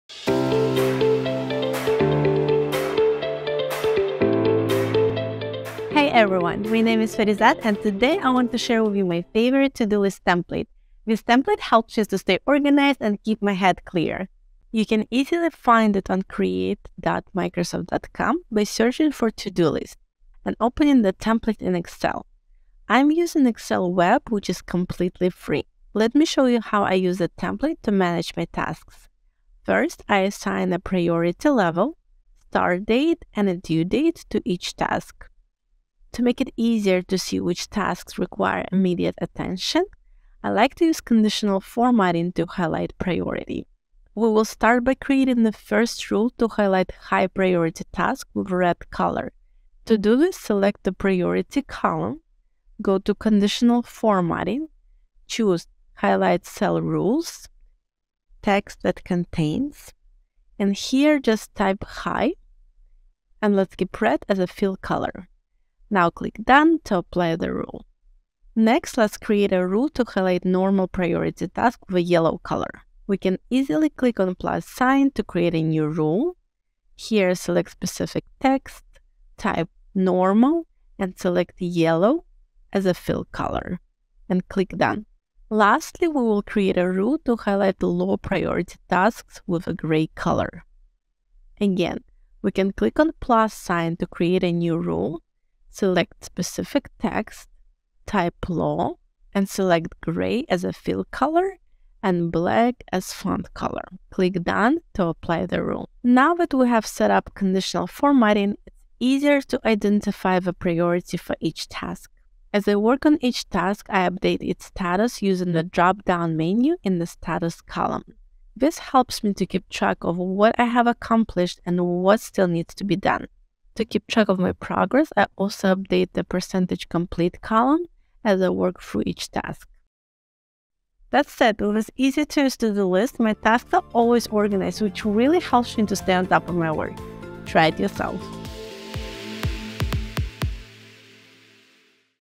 Microsoft Create: Create a to-do list in Excel
Using a to-do list can help keep you organized. Learn how you can use this Excel template to manage your tasks.

► Design unique and beautiful content—social posts, videos, presentations, flyers, and more with Microsoft Create